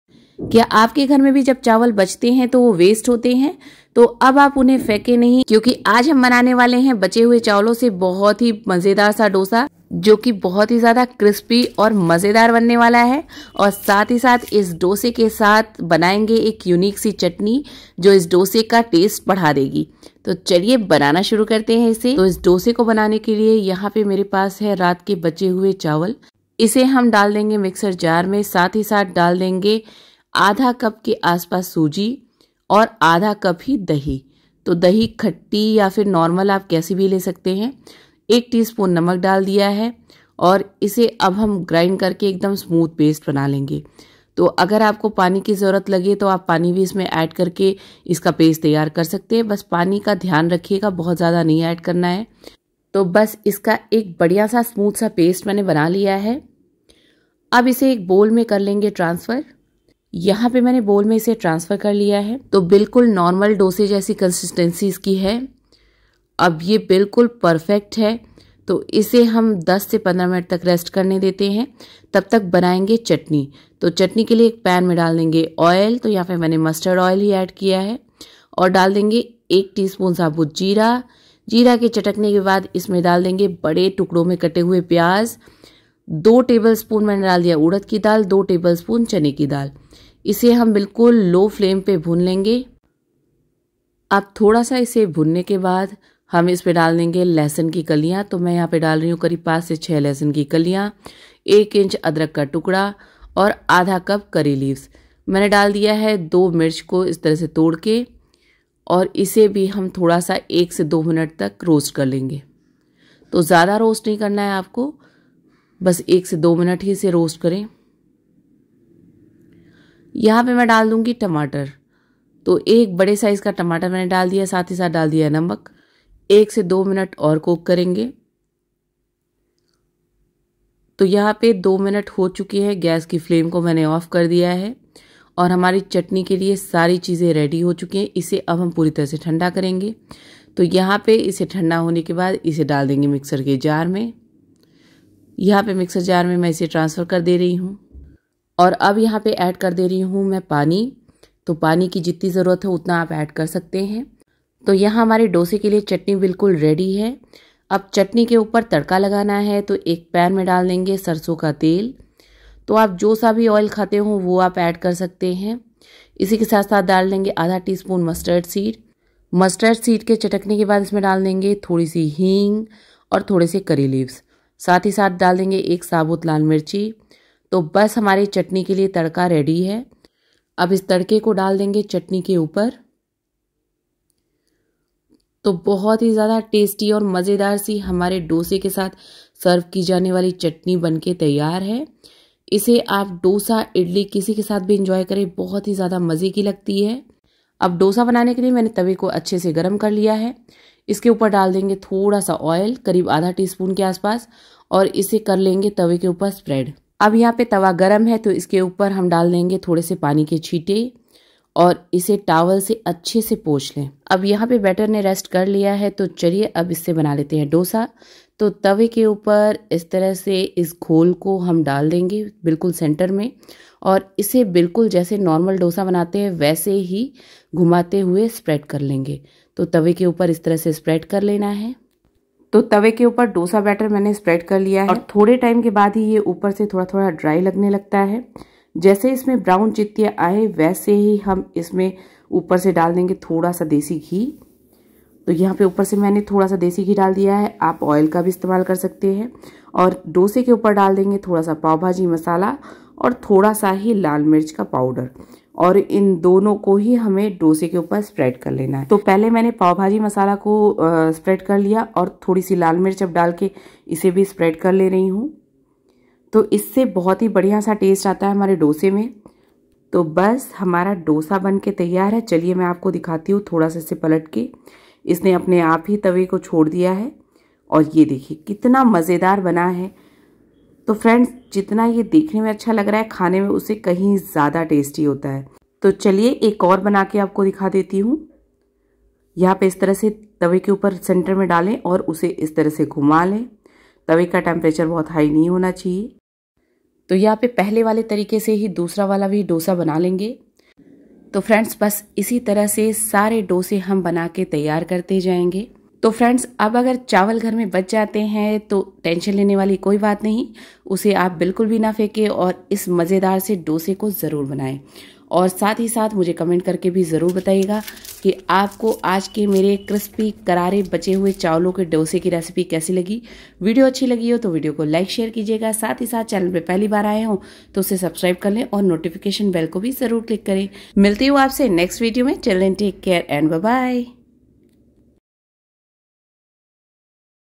बचे हुए चावल के डोसे की रेसिपी देखने के बाद चावल जानबूझकर बचाएंगे | leftover rice dosa | dosa chutny
बच्चे हुए चावल से बनाये क्रिस्पी डोसा | leftover rice recipe | tasty dosa chutny recipe |

Ingredients,

For dosa,
1 cup leftover rice
1/2 cup semolina
1/2 cup curd

For chutney,

1 tsp cumin seed
1 tbsp oil
1 onion
1 tomato
2 tbsp chana dal
2 tbsp urad dal
1/2 cup curry leaves
5-6 garlic cloves
1 inch ginger
1 pinch asifotida
1 tsp salt

For tadka,
1 tbsp oil
1 tsp musturd seed
4-5 curry leaves
1 red chilli